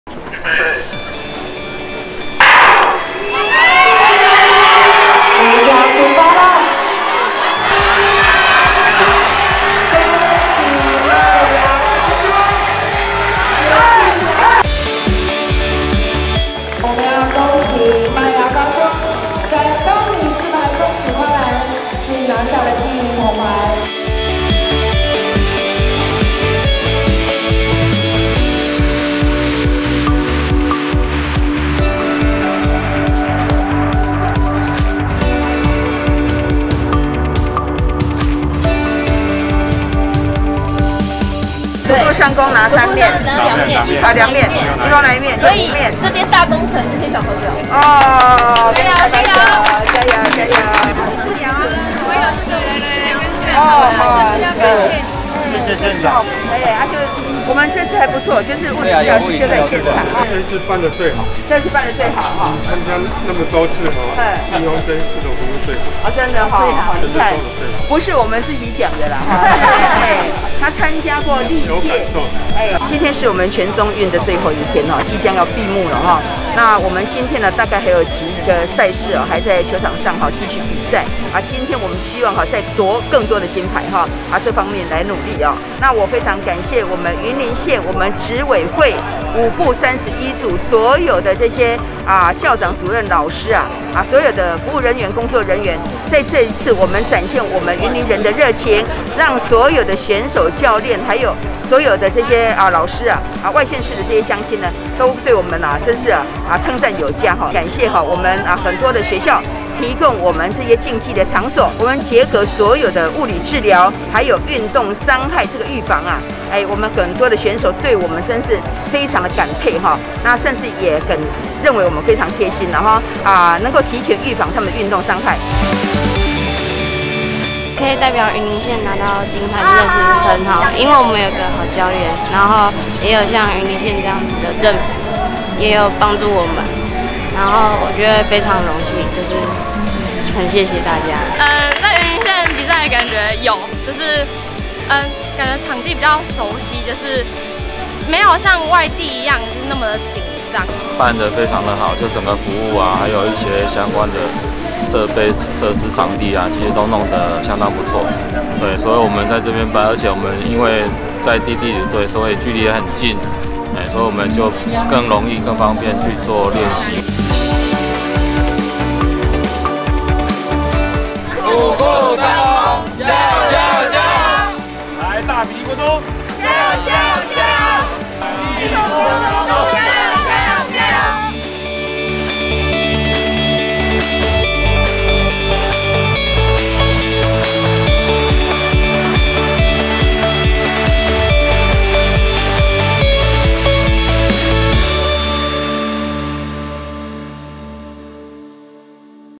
110全中運最後一天 縣長張麗善巡視場地表達感謝
110全中運賽事接近尾聲，比賽最後一天，雲林縣長張麗善特地到各場地，感謝工作人員們的協助與辛勞，也幫選手們加油打氣，雖然賽程已進入最後一天，但雲林縣的選手們依然非常努力，拼命為自己的家鄉奪下勝利的獎牌，這樣的拚勁實在讓人感動。